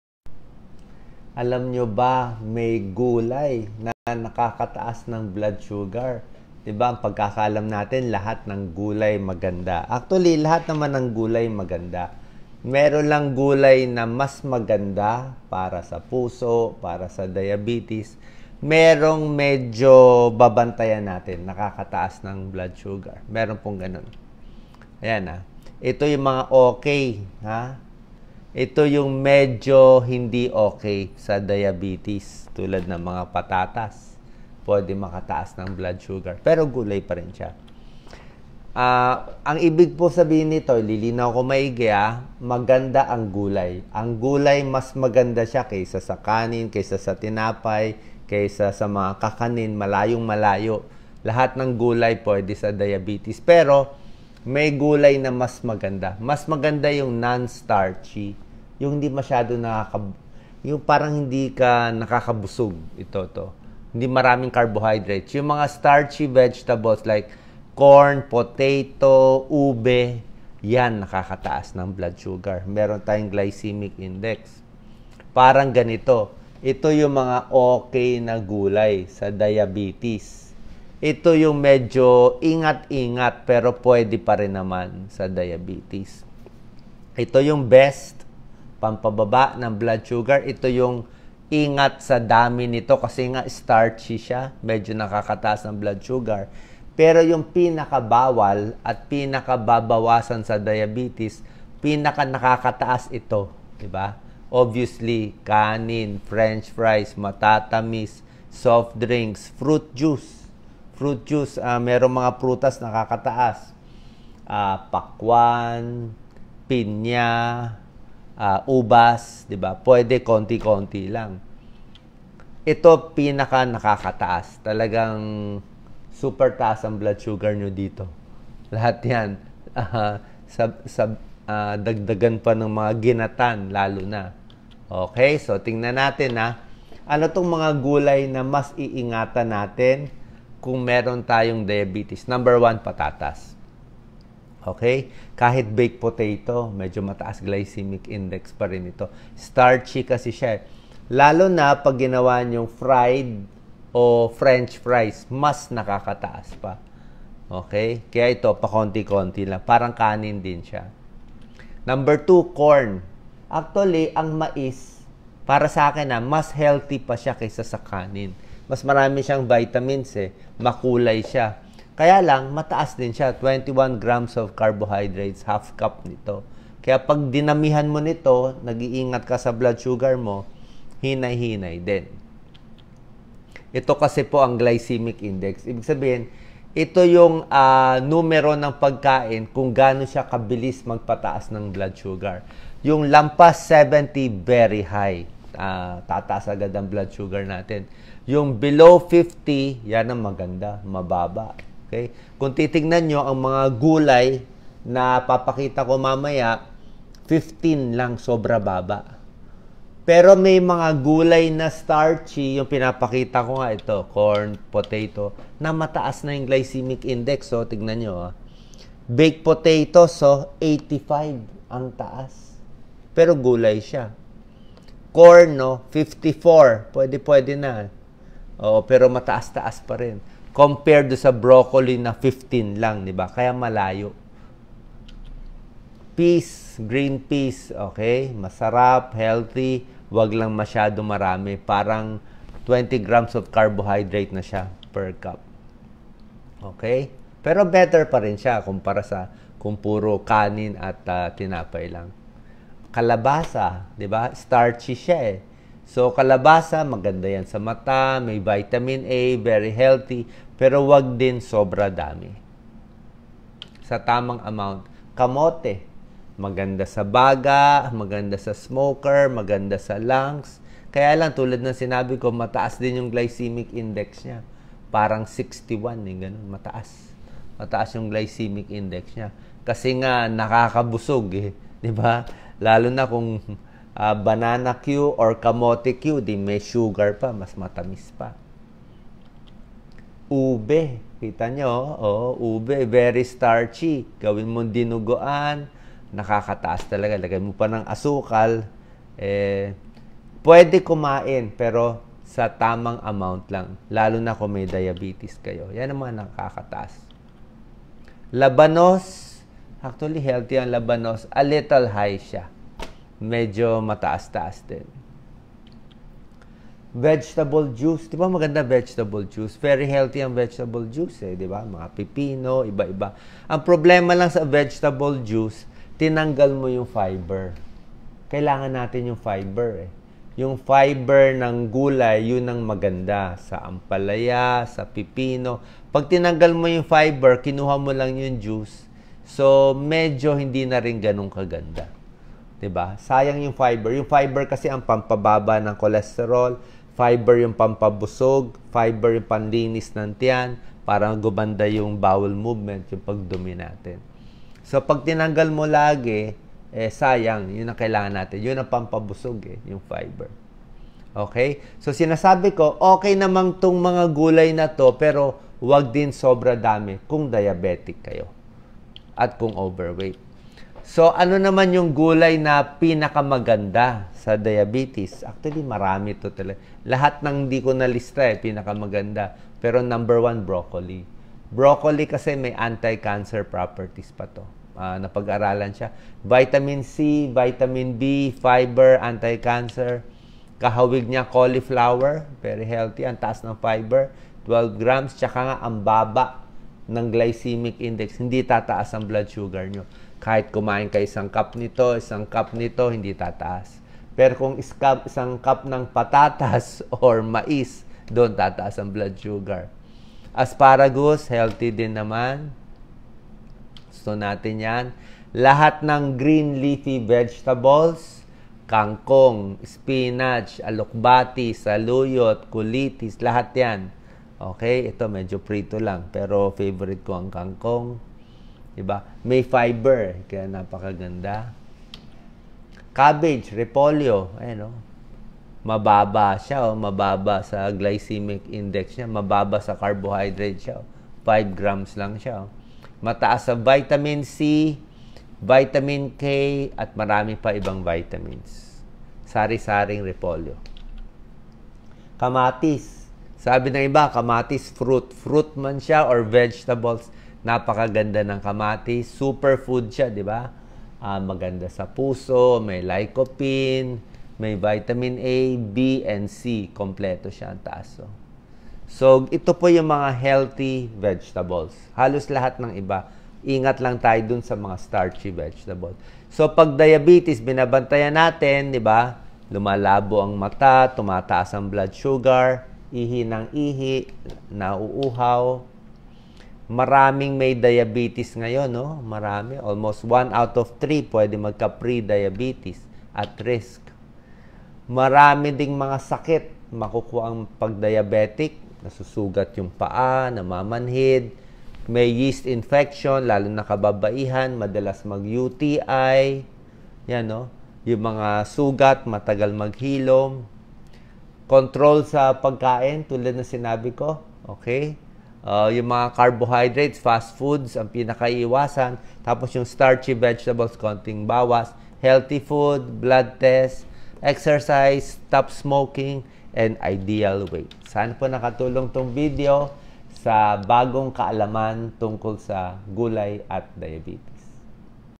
7 Gulay Nakakataas ng Blood Sugar. - By Doc Willie Ong (Internist and Cardiologist)
7 Gulay Nakakataas ng Blood Sugar.
Alamin ang "Good" at "Bad" Vegetables.